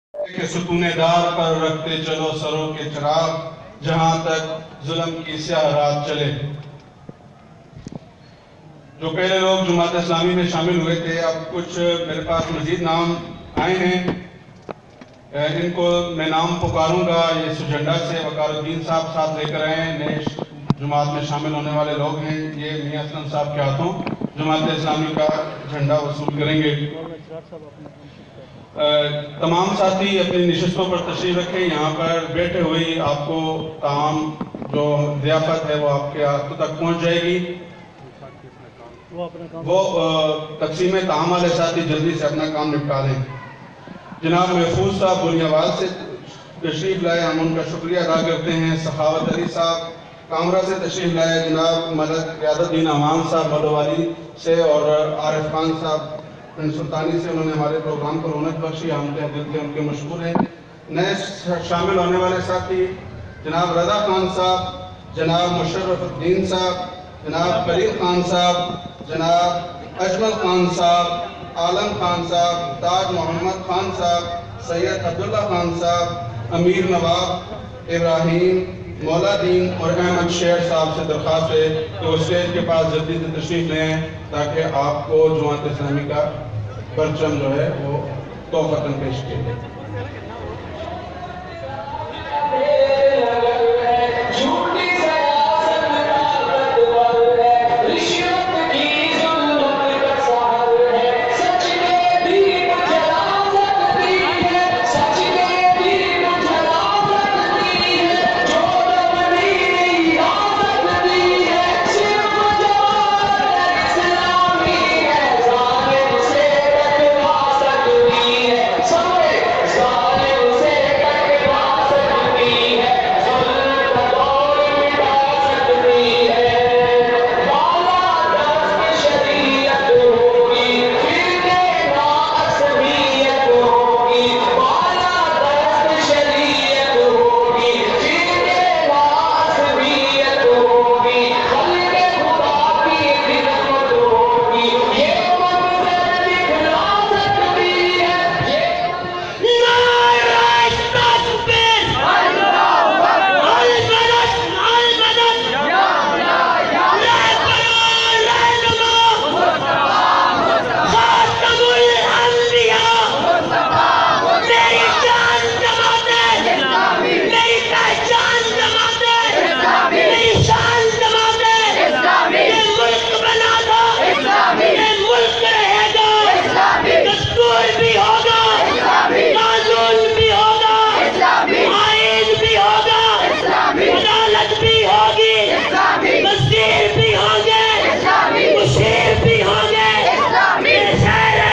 JAMAAT E ISLAMI DISTRICT ATTOCK DARUL ISLAM COLONY
CELEBRAT ED IT'S ANNUAL 80 YEAR'S CONVENTION AUGUST 26**2020